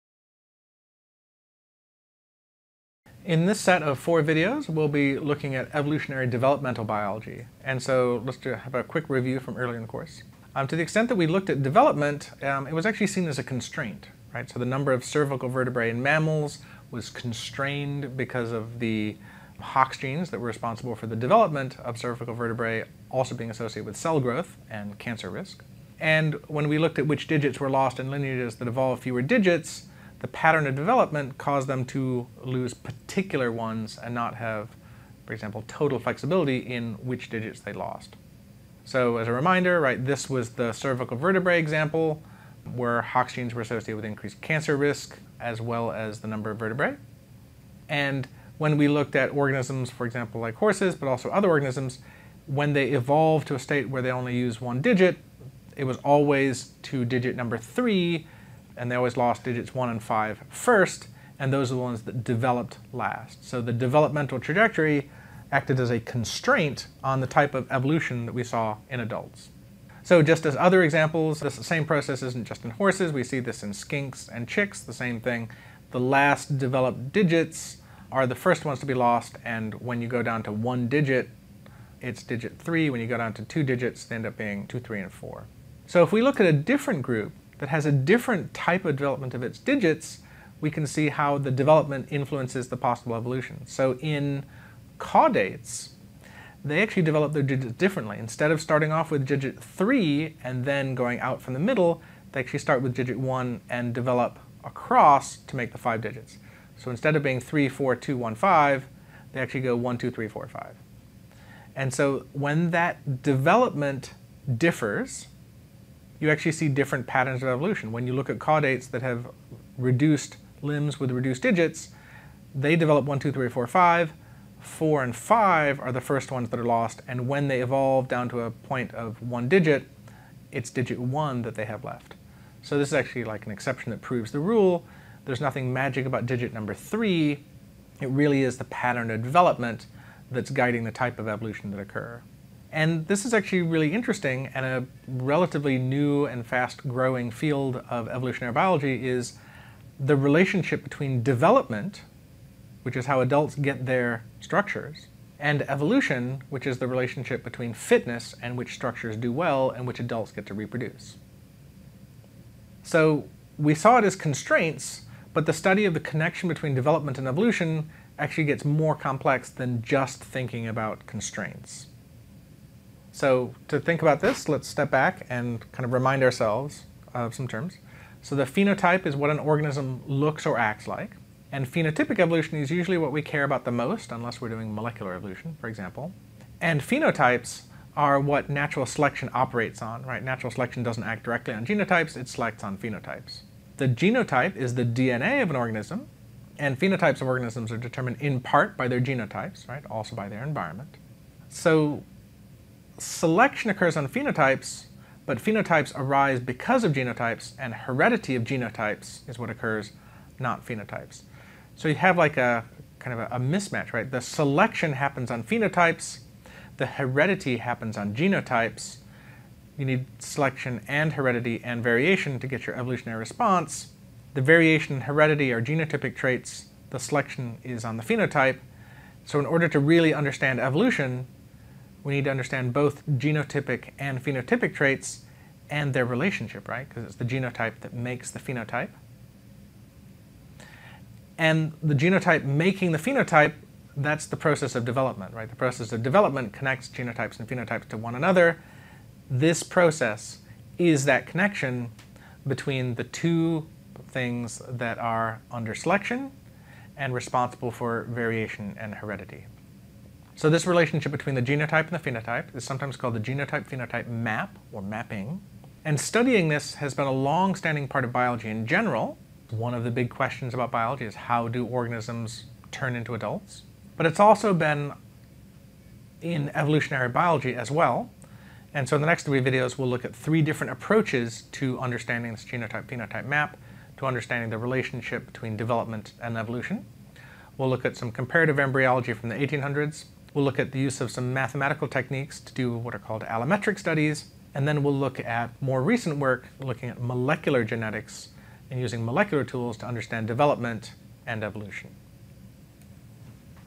Evo-Devo (1), introduction.
This video talks about how developmental biology and evolutionary biology can be used together to better understand aspects of each other. Three basic approaches to this field are introduced: comparative embryology, allometry, and molecular approaches.